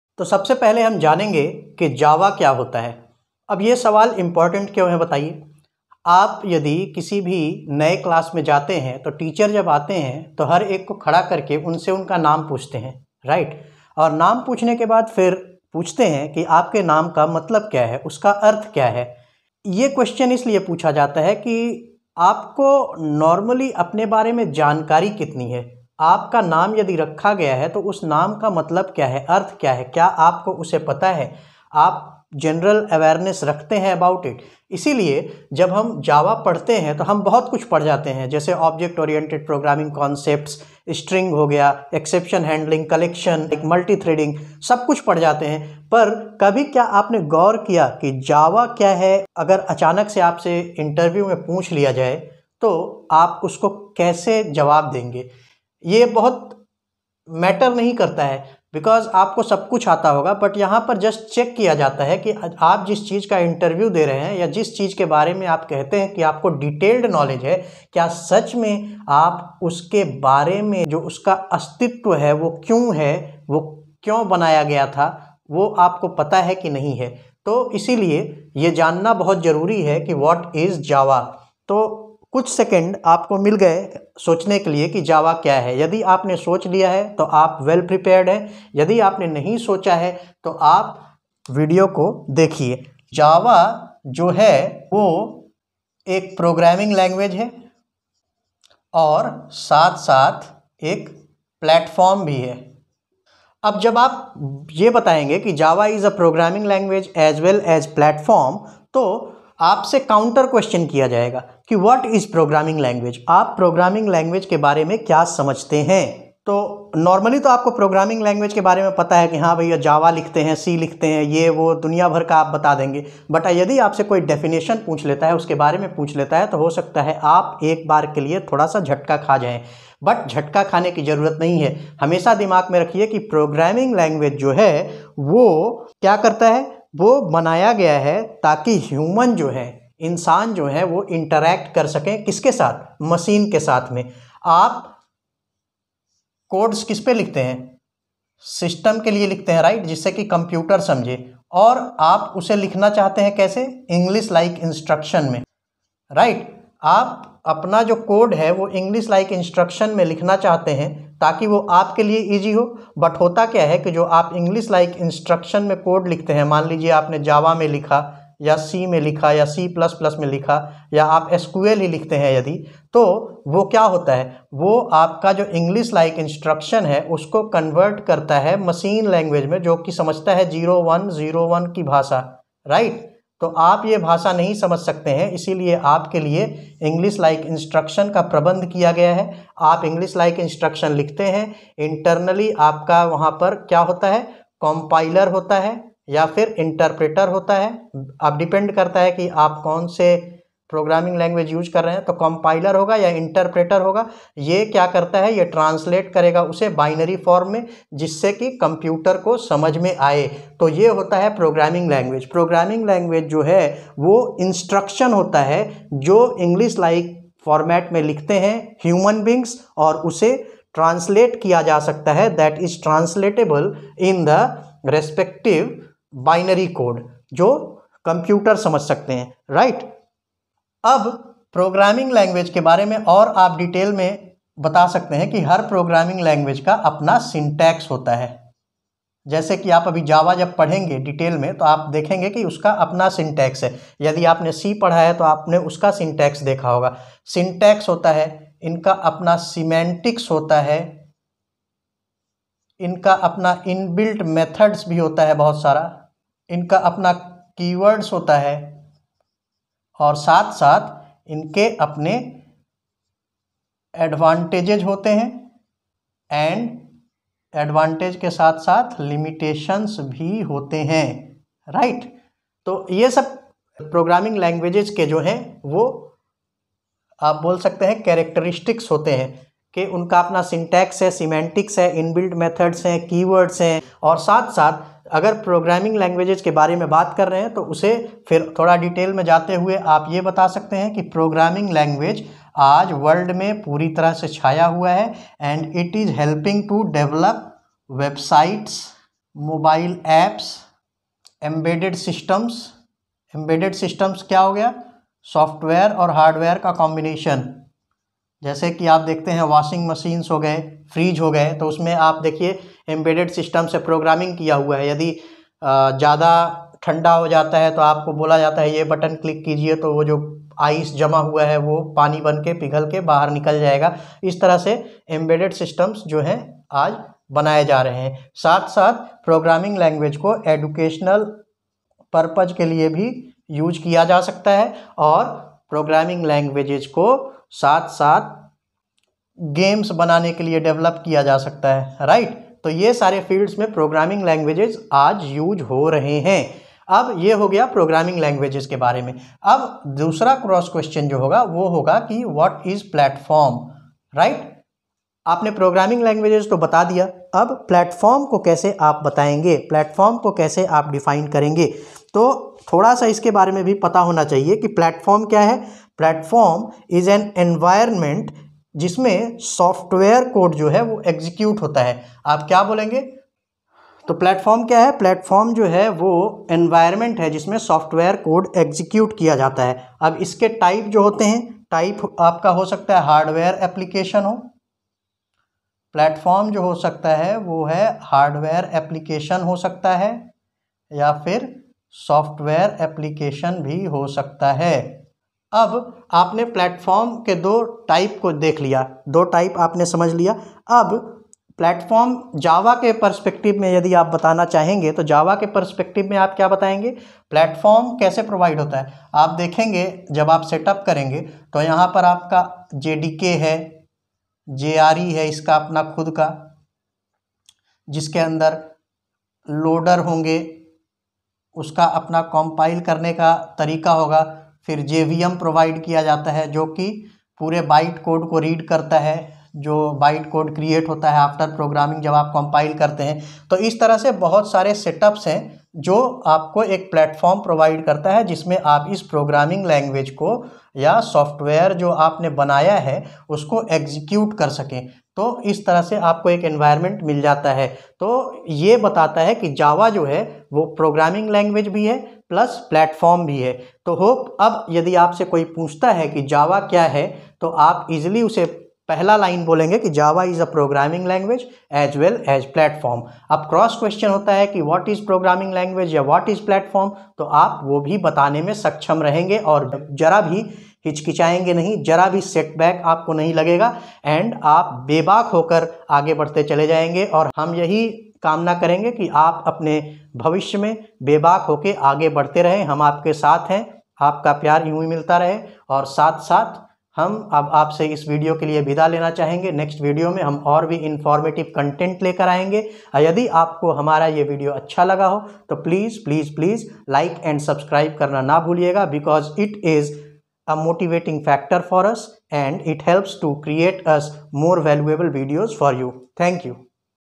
What is java in Hindi? | What is programming language? | What is platform?
Important timelines:
why this question is important? 00:00
what is java? 01:32
what is programming language? 01:52
what is platform? 05:58